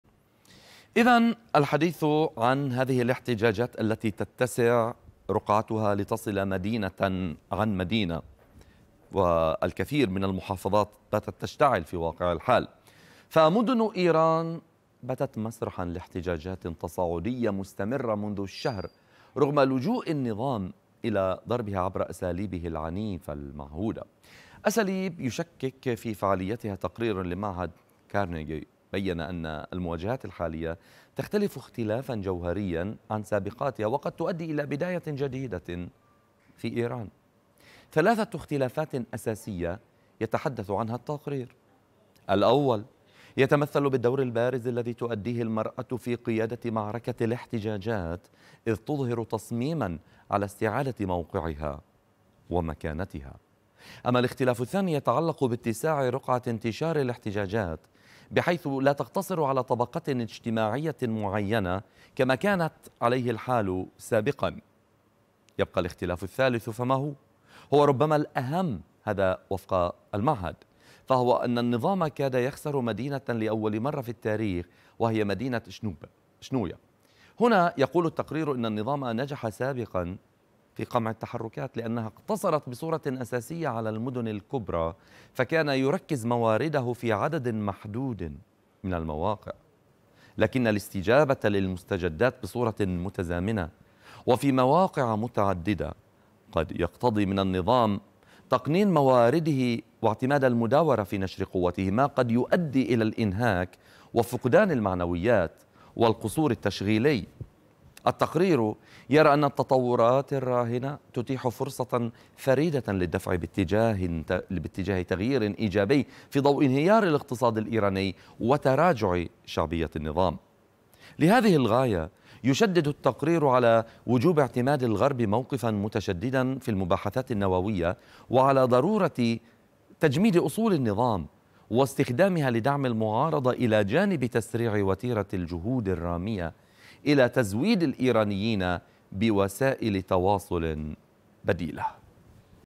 3 اختلافات في مظاهرات مهسا أميني عن سابقاتها بإيران
أبرزها أن النظام كاد يخسر مدينة لأول مرة في تاريخه.. تقرير لمعهد "كارنيجي" يوضح أهم اختلافات مظاهرات مهسا_أميني عن سابقاتها بـ